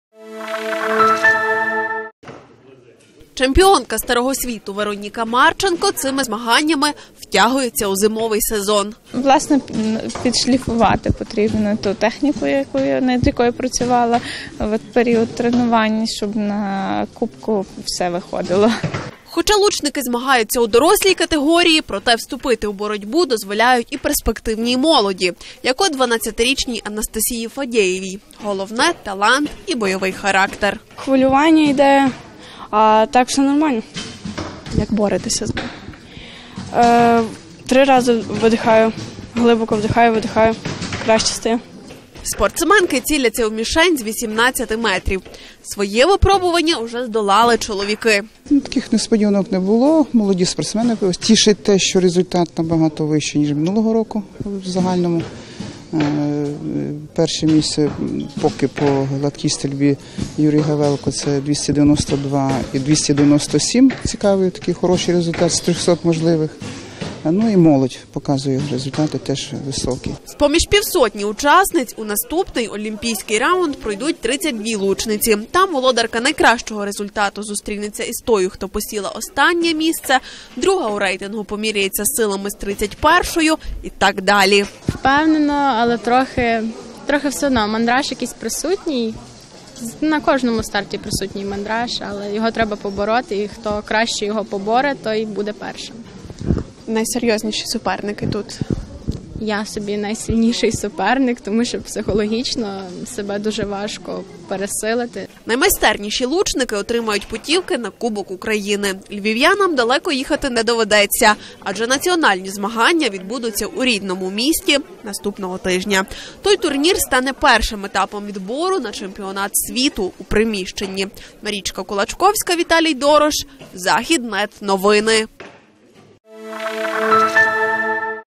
Львівські лучники змагаються за перепустки на Кубок України
Львівські лучники змагаються за Кубок області серед дорослих спортсменів. За зброю взялися півсотні учасників. Чоловіки уже здолали своє випробування, тепер за зброю взялися жінки. Хоча лучники змагаються у дорослій категорії, проте вступити у боротьбу дозволяють і перспективній молоді, як от 12-річній Анастасії Фадєєвій. Дівчина зізнається: «Трохи хвилююся, але борюся з цим. Три рази вдихаю-видихаю, і стає легше». Серед учасниць – чемпіонка Європи Вероніка Марченко, яка цими змаганнями втягується у зимовий сезон. Спортсменка зазначила: «Треба відшліфувати техніку, над якою я працювала у період тренувань».

З-поміж півсотні учасниць у наступний, олімпійський раунд проб`ються 32 лучниці. Там володарка найкращого результату зустрінеться із тою, хто посіла останнє місце. Друга у рейтингу поміряється силами з 31-тою і так далі. Лучниця Ірина Заставна розповіла: «Впевнено почуваюся, але все одно є трохи мандраж. Але його треба побороти. І хто краще це зробить, той буде першим. Я собі найсильніший суперник. Тому що психологічно себе дуже важко пересилити». Наймайстерніші лучники отримають путівки на Кубок України. Львів`янам далеко їхати не доведеться, адже національні змагання відбудуться у рідному місті. Той турнір стане першим етапом відбору на чемпіонат світу у приміщенні.